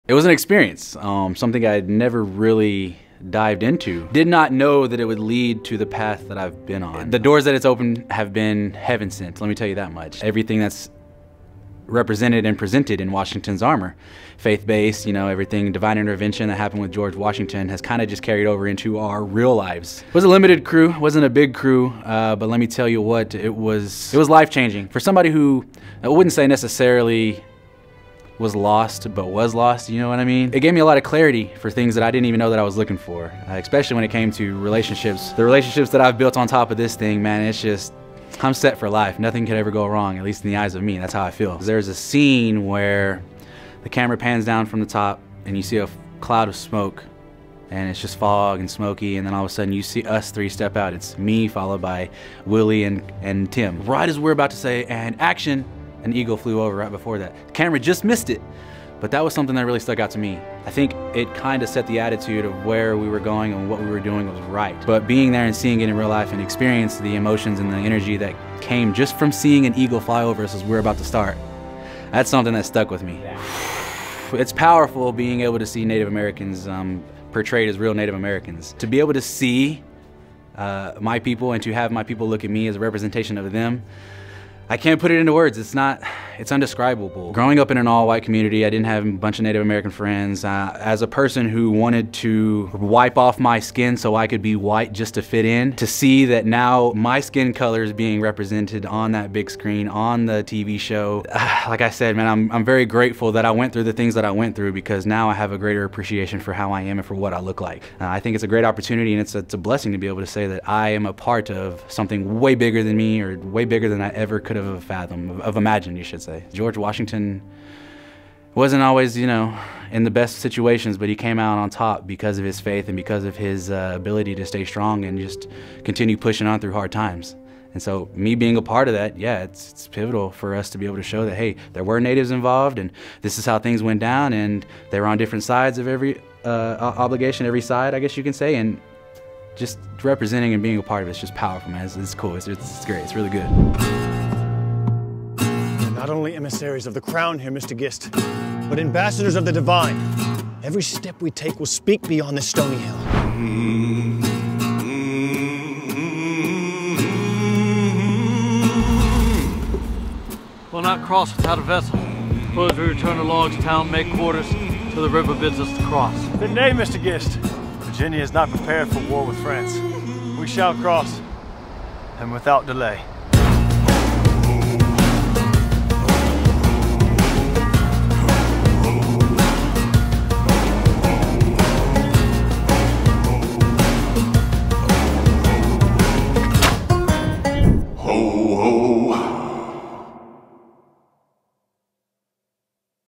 Native American ACTOR Jeremey Gauna Interview
Jeremy Gauna, a Choctaw Indian, describes his role in Washington's Armor, his experience as an actor, and what it means to be a Native American working in the film industry.